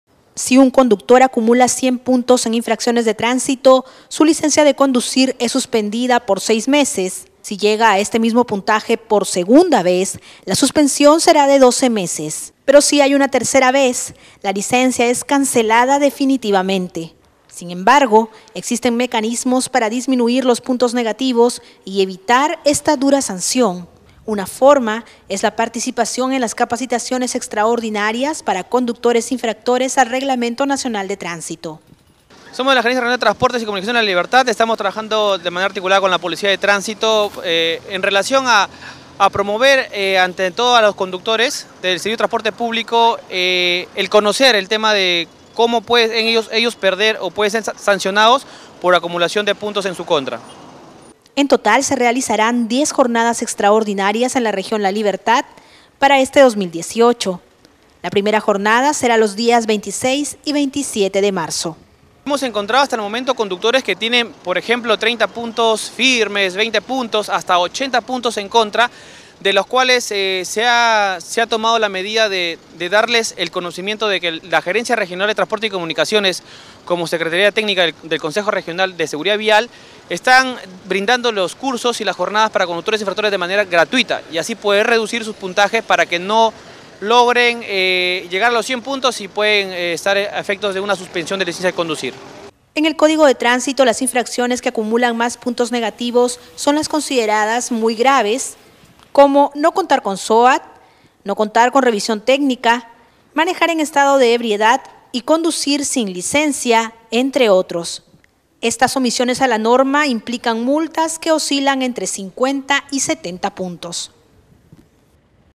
NOTA CAPACITAN A CHOFERES
El área de seguridad vial de la Gerencia Regional de Transportes brindará capacitación a los choferes que tengan infracciones leves. La primera jornada se desarrollará el 26 y 27 de marzo.